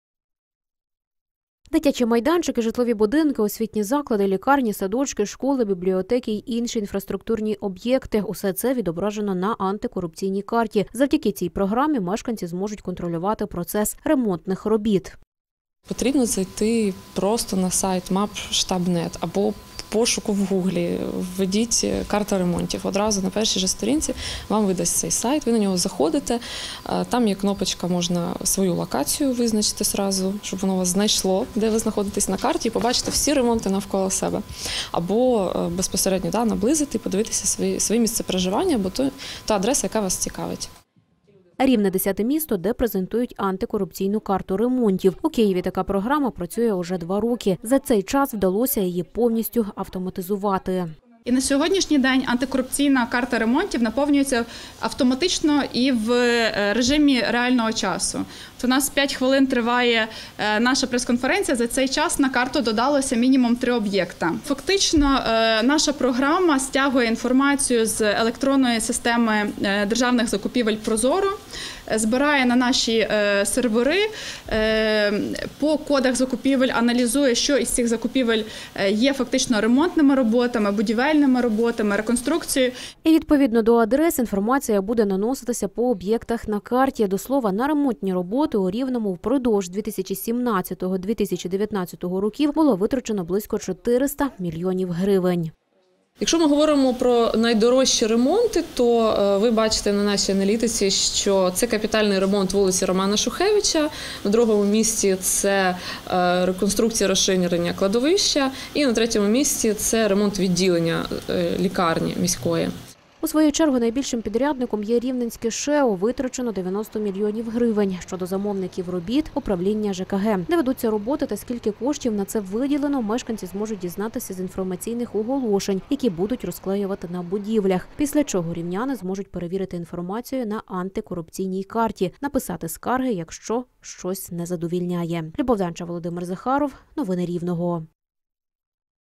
Гроші під контролем: рівняни перевірятимуть якість ремонтів через Інтернет
Ремонт під контролем громадськості. Скільки коштів виділено на ваш будинок, майданчик? Чи виконані роботи якісно, згідно з графіком - усе відтепер можуть перевірити мешканці на Антикорупційній карті.